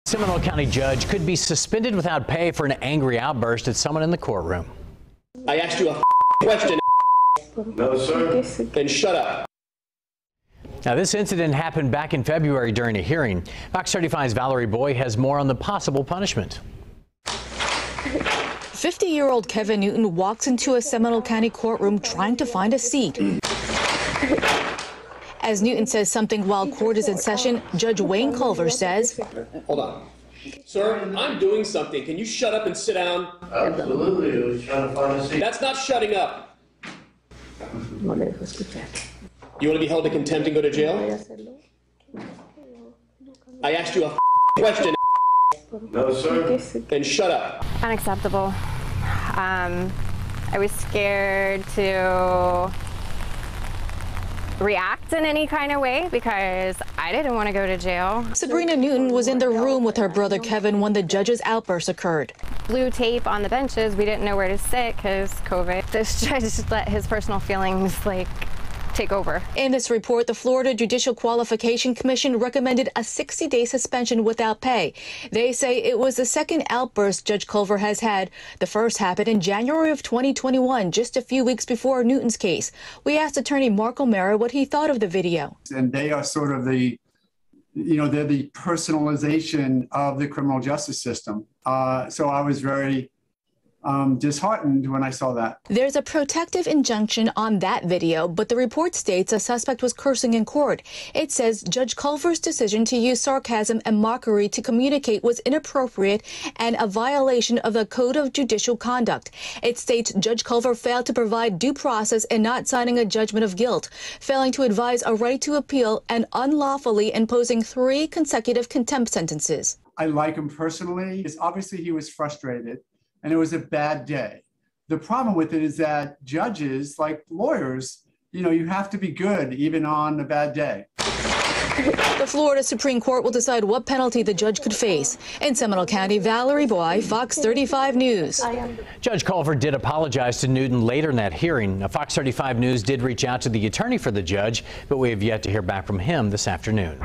Florida judge has angry outburst at man looking for a seat in courtroom
A judge in Seminole County continues to be seated on the bench, as the Florida Supreme Court decides if it will reprimand him for two different outbursts.